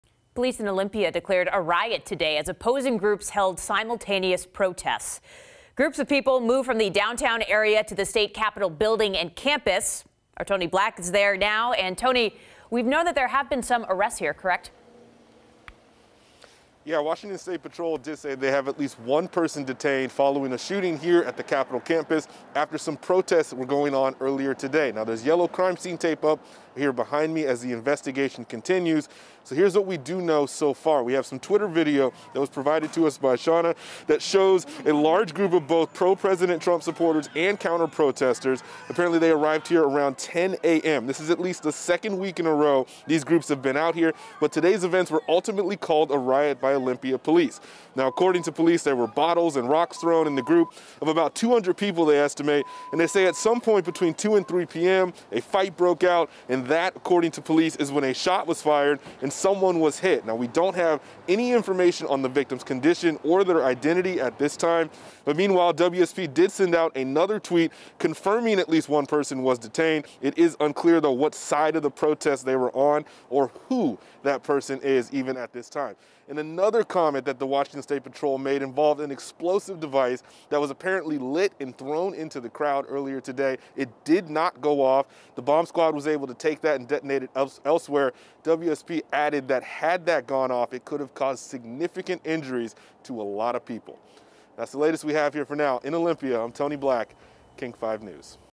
1 person shot after opposing protests in Olympia turn violent
Police in Olympia say one person is in custody following a shooting at a violent protest between two heavily armed groups near the Capital building in Washington state.

The Thurston County sheriff's office says they arrested one person following the shooting Saturday afternoon but didn't immediately release details.